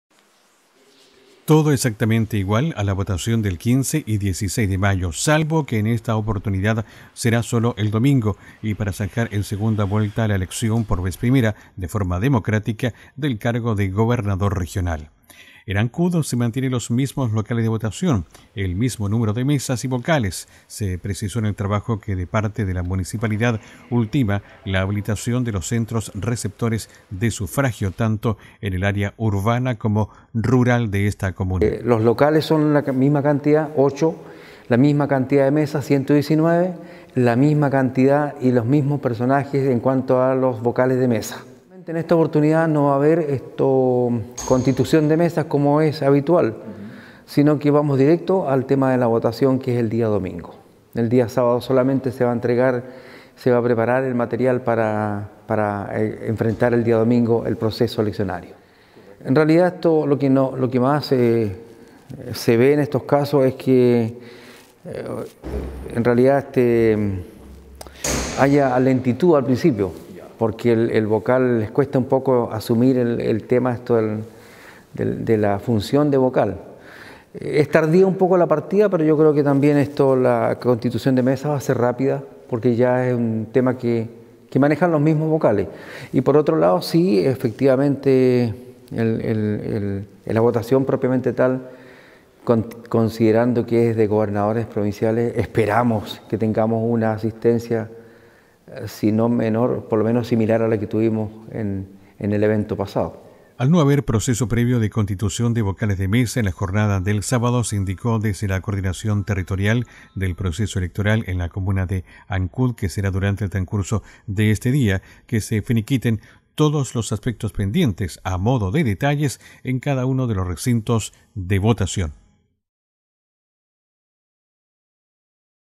JORNADA ELECTORAL DOMINGO 13
En Ancud habilitados los locales de votación para segunda vuelta de gobernadores regionales